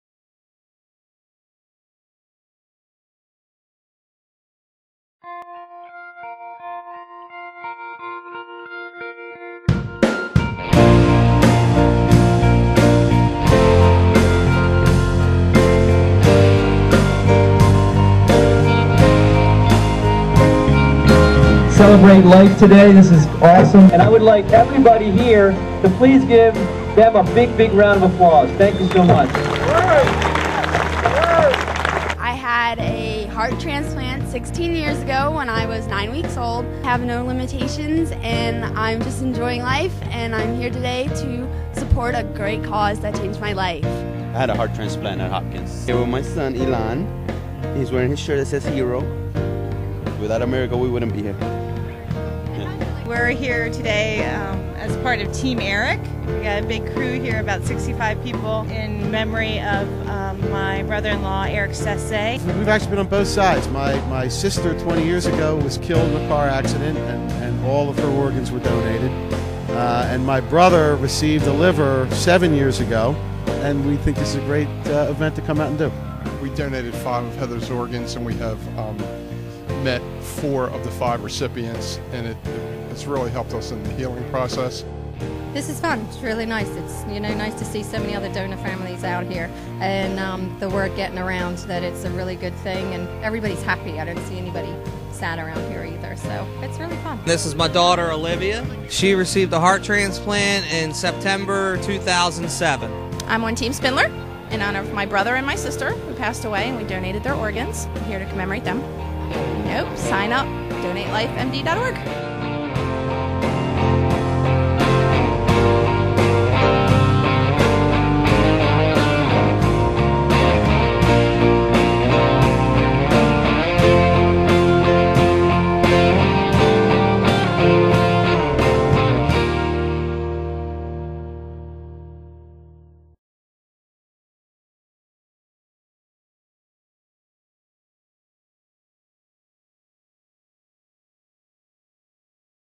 2012 Donate Life Family Fun Run
The Donate Life Family Fun Run is a non-competitive 5K run and 1K walk to raise awareness for organ, eye and tissue donation and celebrate Maryland's organ and tissue donors and their families. The Living Legacy Foundations welcomes any and all supporters of organ, eye and tissue donation to participate in this family friendly event on Saturday, September 22, 2012.

Proceeds from the Donate Life Family Fun Run benefit the Living Legacy Foundation of Maryland to further the organization's mission of providing advocacy, family support, comprehensive public and professional education, and commitment to enhancing the organ and tissue donation process.